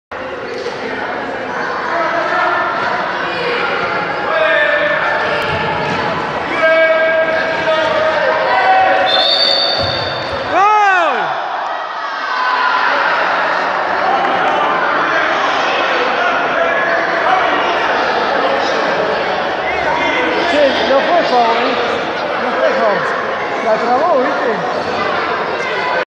21.10.17 GOL MAL ANULADO
Santiago traba claramente la pelota (en ningun momento hace Foul) le pone un gran.pase a chirola quien define cruzado, pero el referí interpreta (MAL) que fue foul de Santiago .'